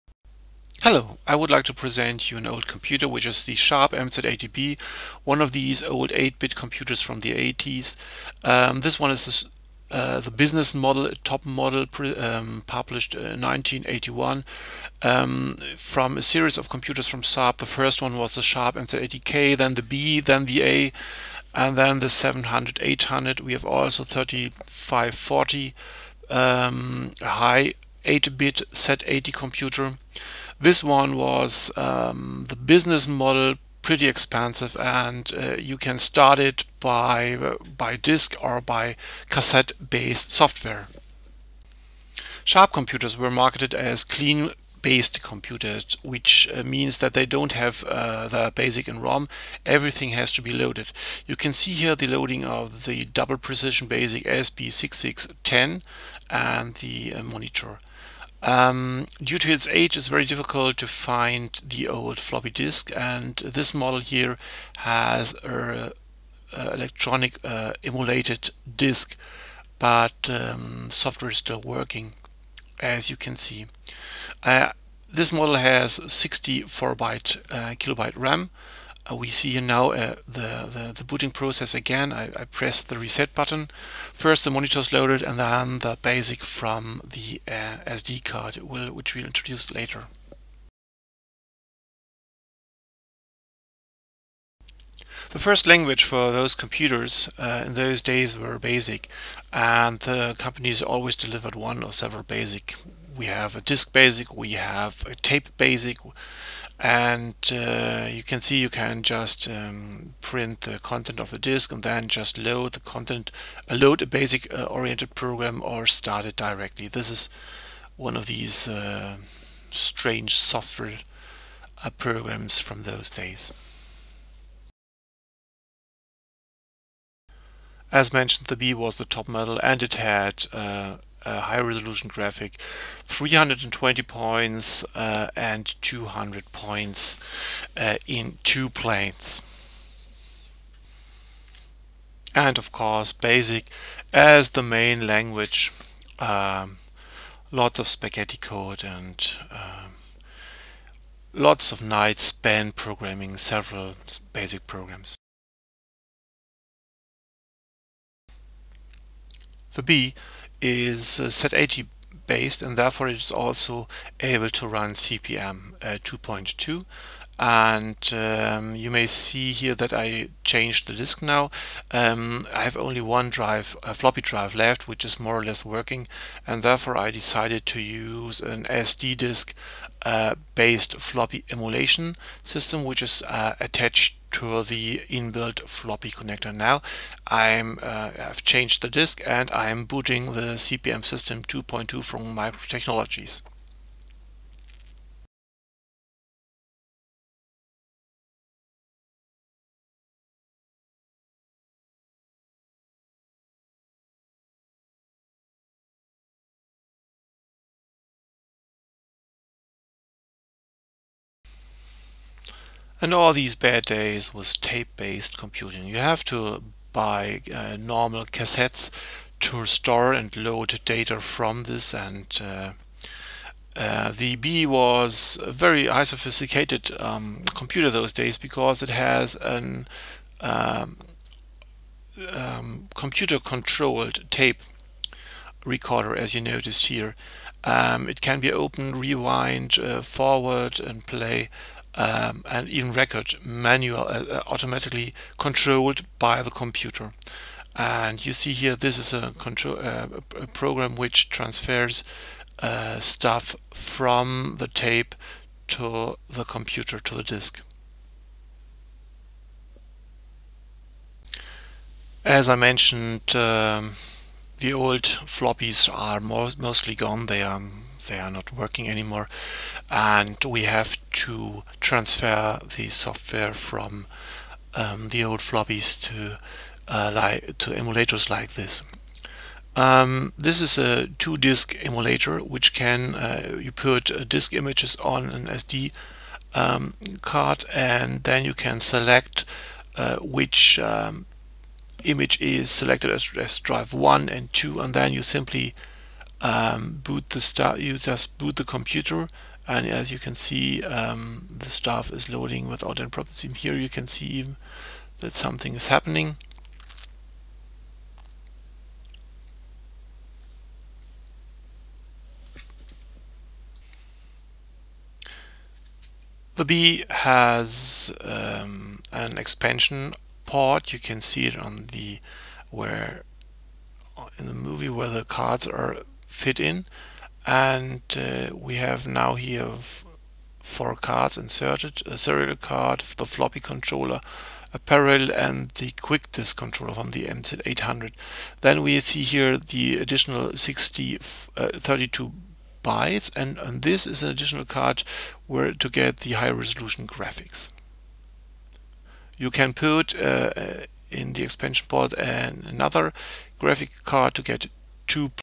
Sharp MZ80B - an introduction of an old Z80 computers
The Sharp MZ80B - introduced 1981 as a clean computer.